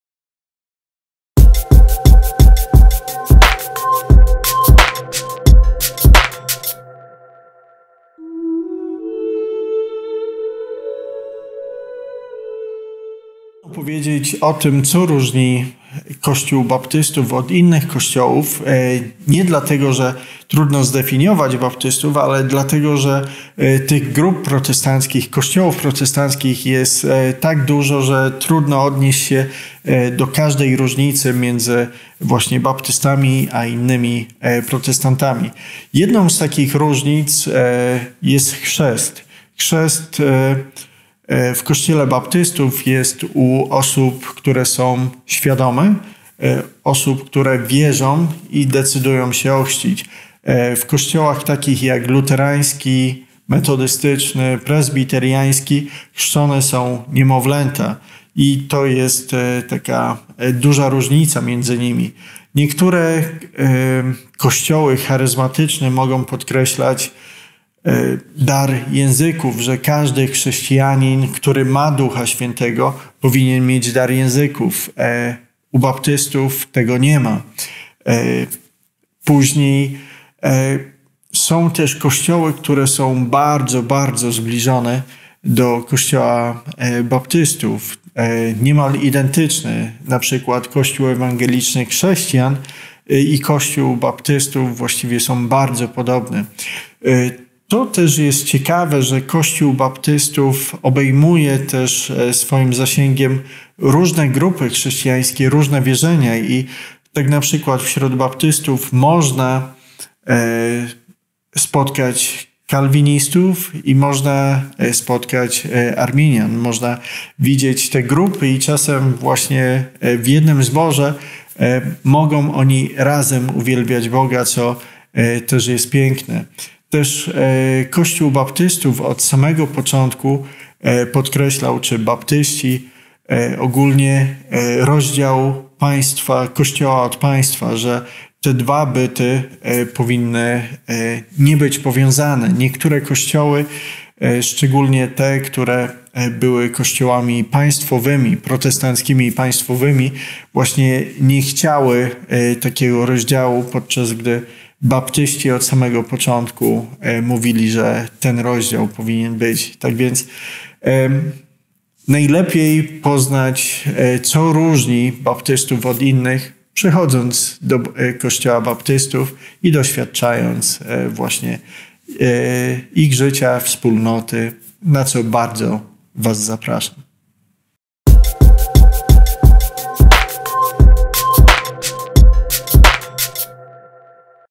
JAK WYŻEJ | Co odróżnia baptystów od innych protestantów? | Baptyści Szczecin
Witamy w naszej serii pytań do Pastora pod nazwą JAK WYŻEJ!

Na dzisiejsze pytanie odpowiada Pastor Dr Robert Merecz z Kościoła Chrześcijan Baptystów w Szczecinie.

Czym baptyści różnią sie od innych protestantów?

Zapraszamy do wysłuchania i komentowania oraz subskrypcji - nowe pytania i odpowiedzi będziemy publikować w każdy wtorek i piątek o 18:00.